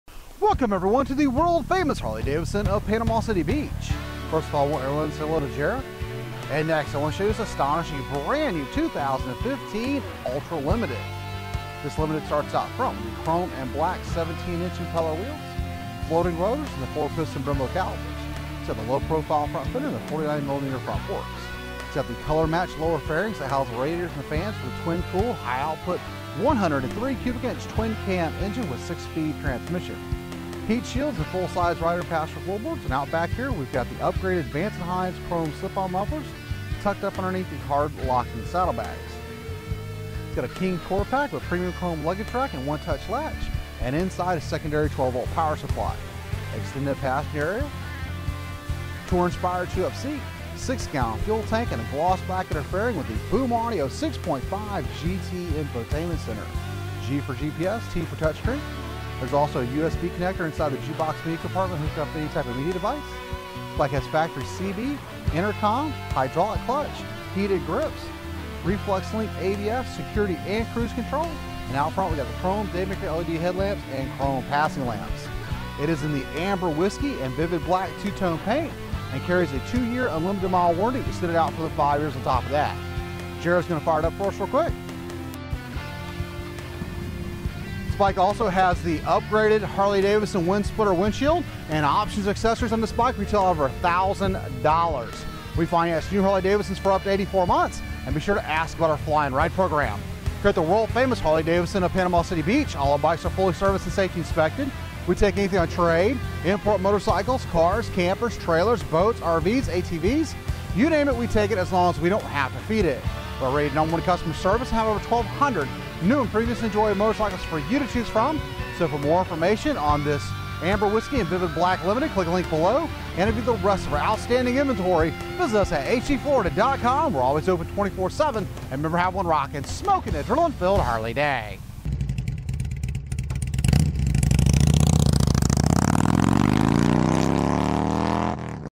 2015 Harley Davidson Ultra Limited Motorcycles for sale Alabama Florida
New 2015 Harley Davidson FLHTK Electra Glide Ultra Limited Motorcycles For Sale in Panama City Beach FL AL USA Contact . For more information on Price, Colors, Specs and Reviews

Featuring: Vance & Hines mufflers, heated grips, ABS, security, cruise control, CB, intercom. Amber Whisky / Vivid Black two tone (accessories and options total over $1000)

PCB
Stock#  C15151.1